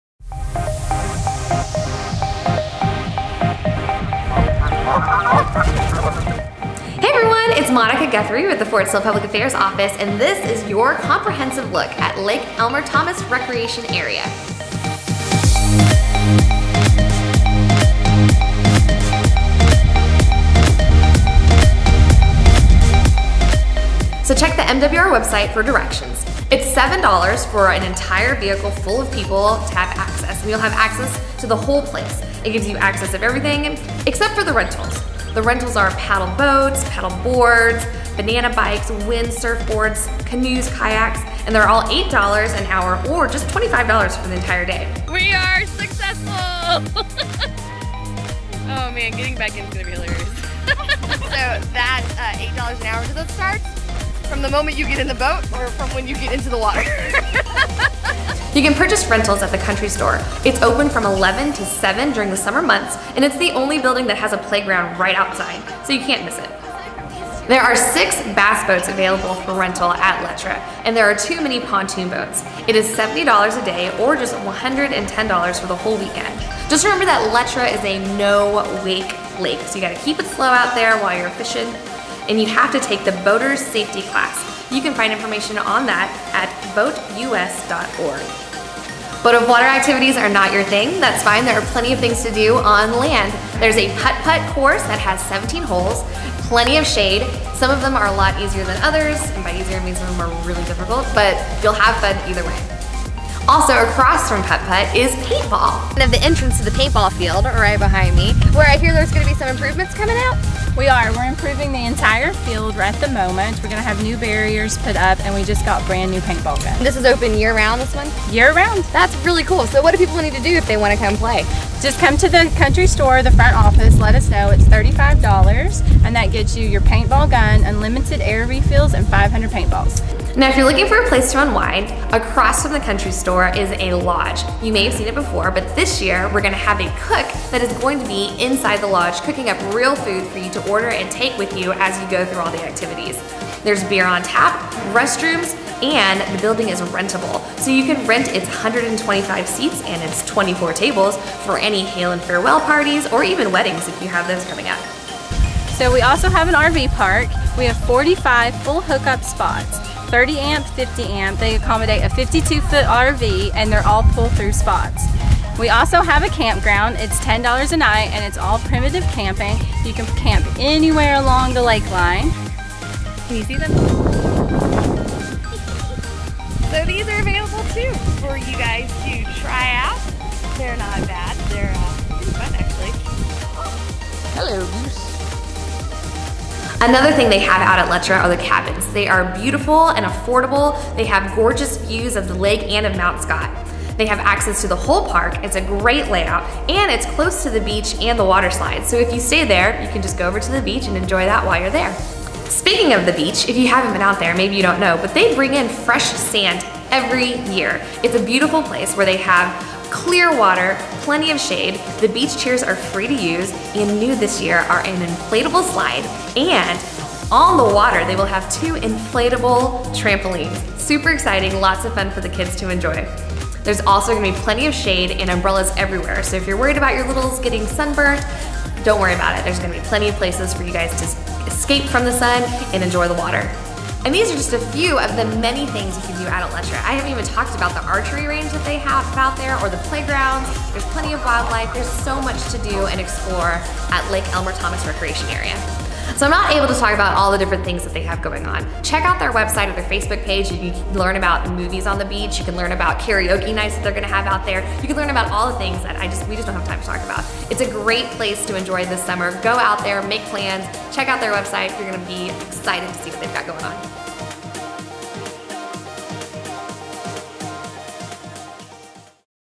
Tour of Lake Elmer Thomas Recreation Area (LETRA)
Don't miss a thing with this full tour of Fort Sill's best kept secret. If you have access to the post and live within the area, you need to go and enjoy this huge outdoor recreation area. This tour gives an in depth look at the facility including prices.